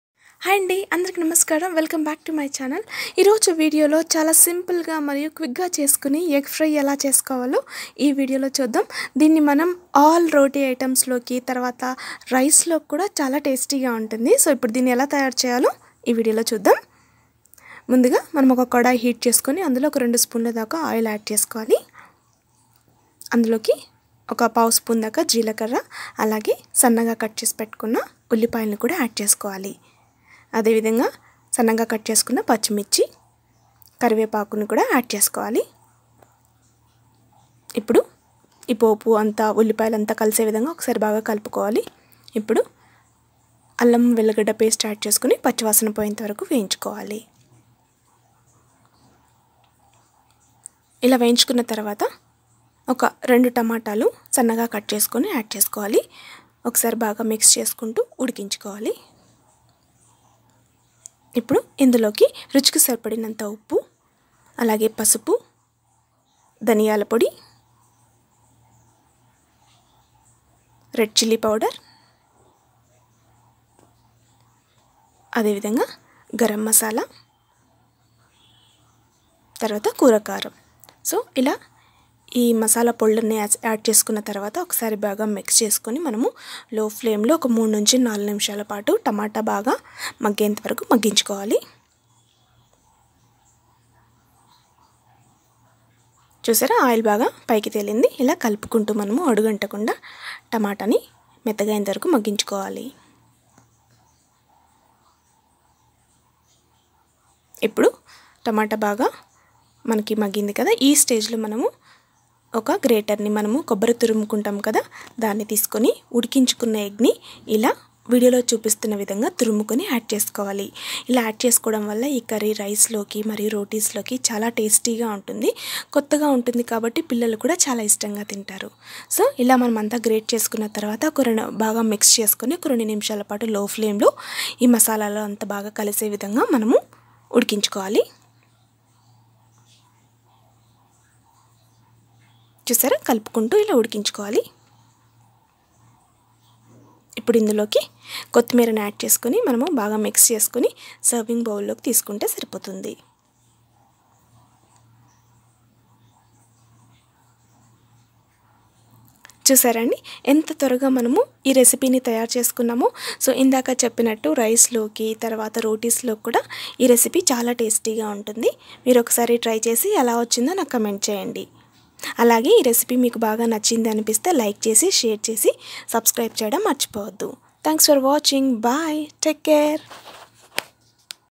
Egg Fry | ఇలా వెరైటీ గ ఎగ్ ఫ్రై చేసుకుంటే టేస్ట్ అదిరిపోతుంది | Egg keema recipe
In this video I've shown tasty egg fry / egg keema recipe.